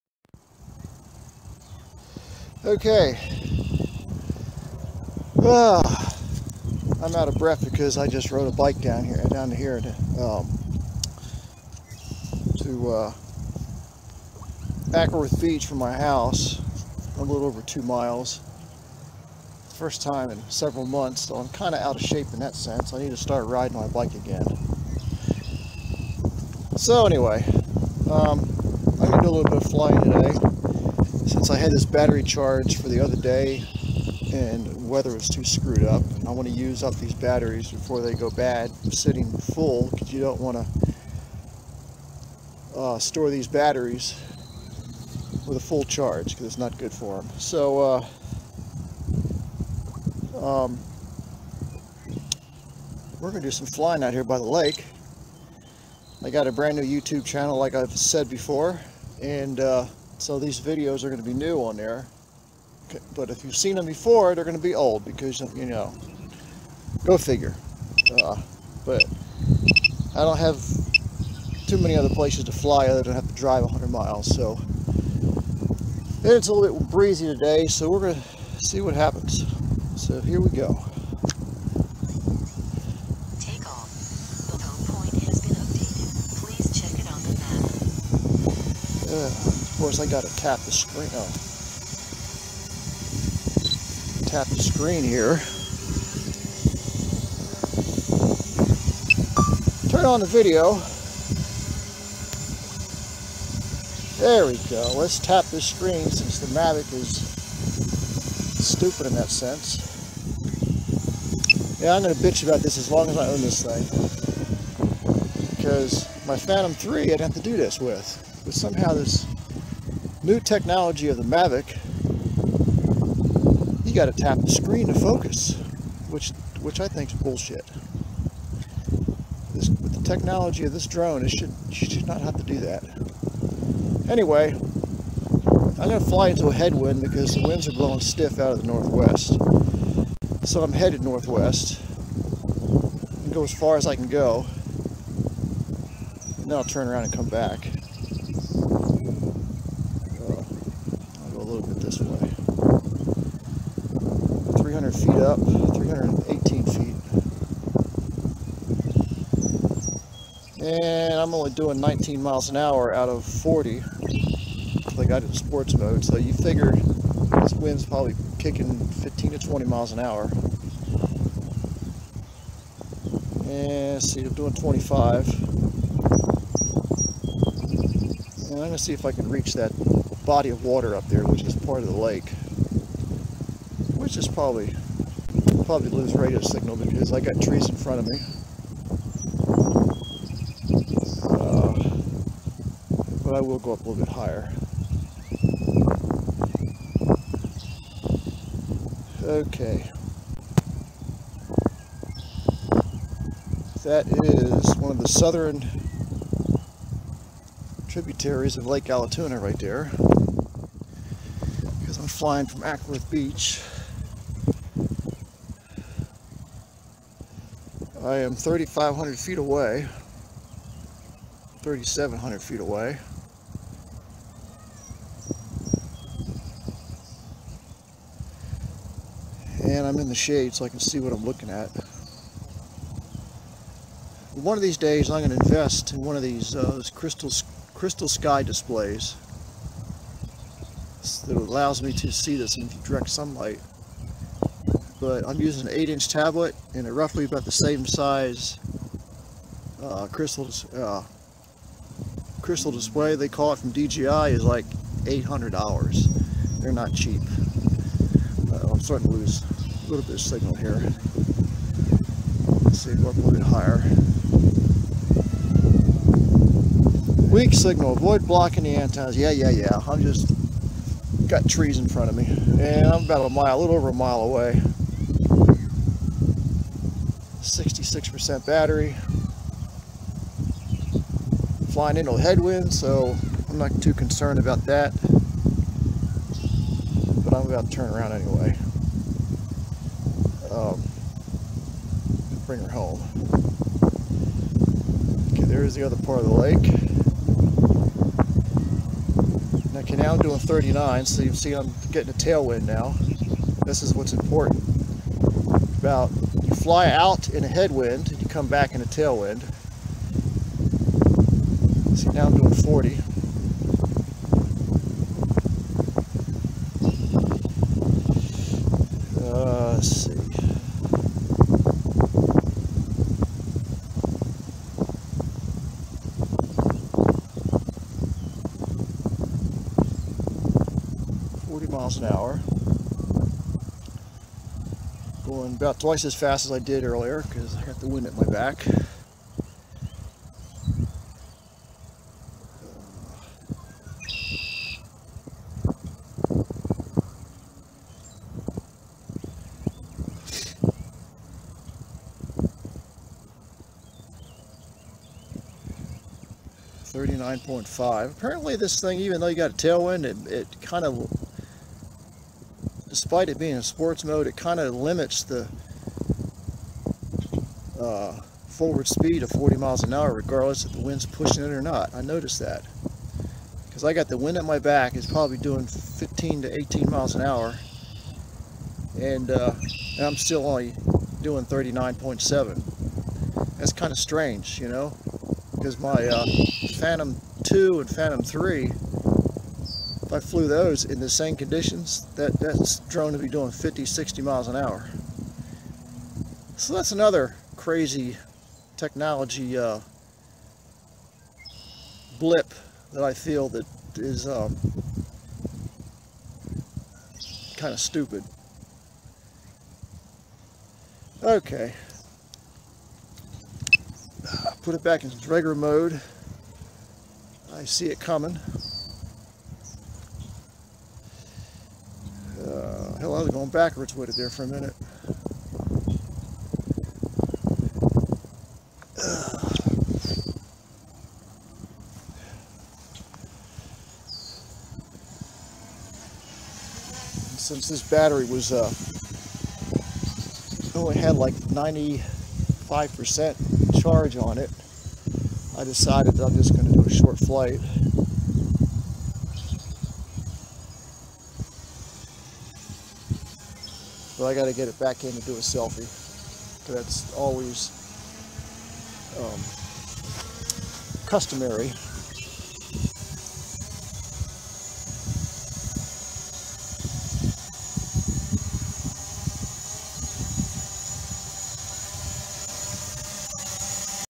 5-13-19 DJI Mavic Pro and dashware
Somehow The dashware Program will work with 4K, but it downgrades it to 1080 even though its set to 4K. Video does not seem as clear.